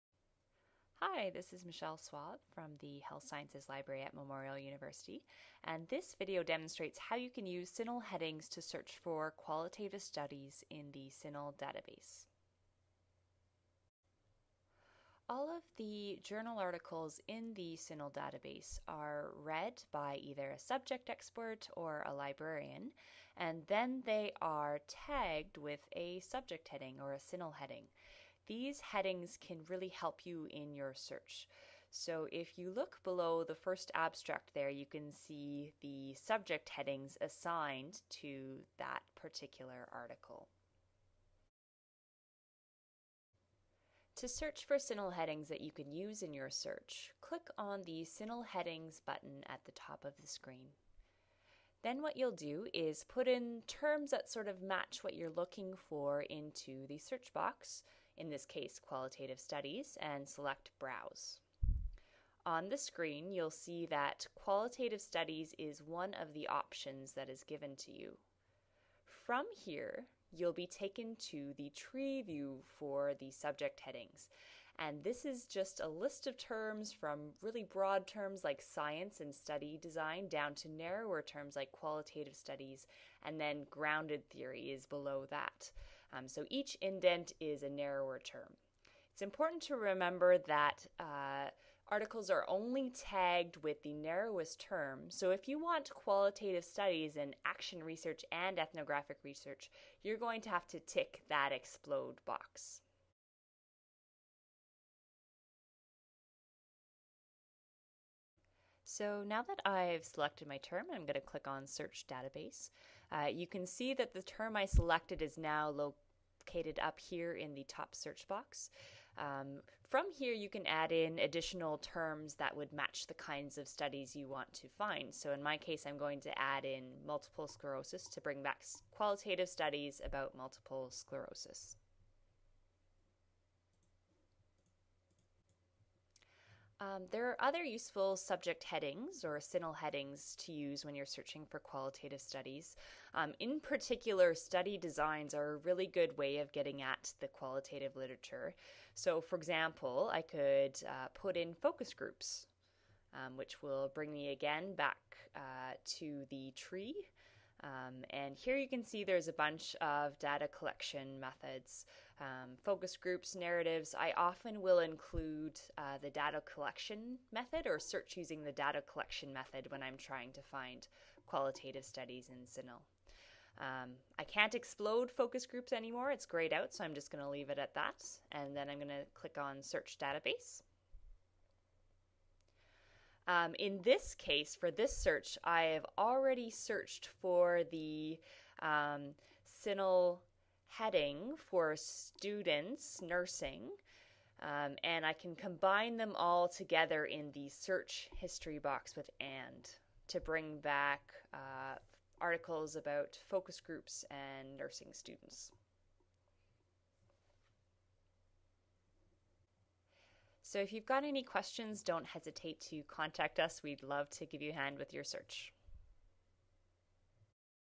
Using CINAHL Headings to Search for Qualitative Studies
Learn how to use subject headings in the CINAHL database to locate qualitative research studies on your topic.

CINAHL is used by Nurses, allied health professionals, researchers, nurse educators and students to find articles in thousands of nursing and allied health journals.

This video was created by Public Services Librarian, Michelle Swab, Health Sciences Library, Memorial University of Newfoundland.